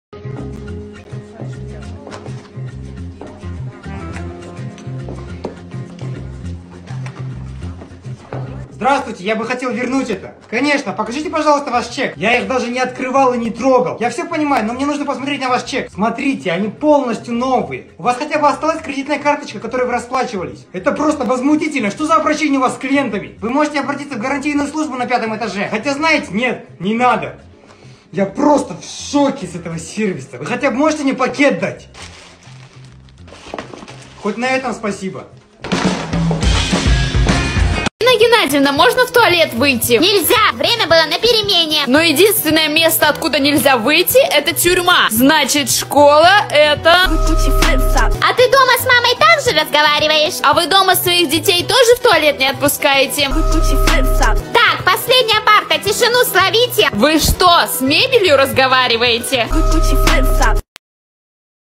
Обхитрил продавца и получил айфон и другую технику Apple бесплатно
Он мечтал об айфоне, но у него не было денег. Тогда он придумал способ как получить его бесплатно